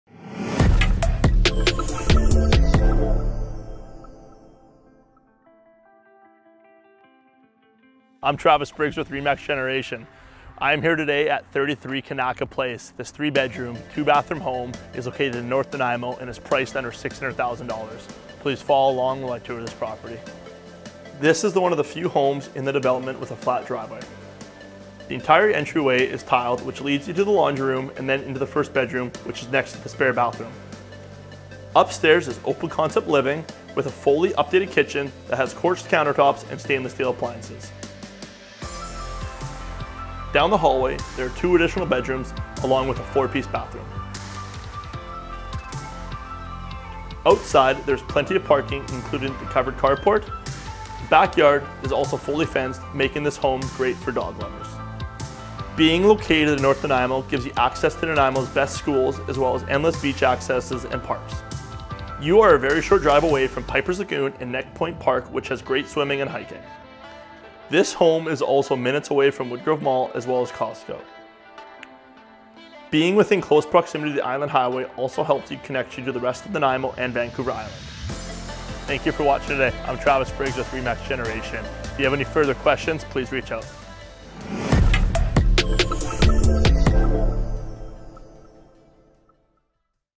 33 Kanaka Place, Nanaimo, B.C. | Travis Briggs PREC*
33 Kanaka Place, Nanaimo, B.C. | Travis Briggs PREC*

This beautifully updated and well-taken care of home is located in a highly desirable area of North Nanaimo.
Key Highlights |

The home is approximately 1600 sq.ft. with 3 bedrooms and 2 bathrooms. The kitchen has been fully updated including new cabinets, quartz countertops, and stainless-steel appliances. Large windows throughout the home allow for natural light. The downstairs of the home is perfect for guests with its own bathroom, bedroom, living area, and French doors to access the back sundeck. There is a fully fenced backyard, perfect for entertaining or a safe place for a four-legged friend. This home is unique by being one of the only homes within its development that has a flat driveway and extra parking. The location is highly desirable not only because of the close proximity to schools and amenities, but also because of the wonderful parks in the area such as Neck Point Park, Piper's Lagoon, and Departure Bay Beach to name a few. The main Island Highway is only a 7-minute drive providing easy access to other parts of Nanaimo and destinations across Vancouver Island. BC Ferries and floatplane services are also a short drive to allow convenient access to the Lower Mainland.

Sean McLintock PREC with RE/MAX Generation

(All measurements and data are approximate and should be verified if important).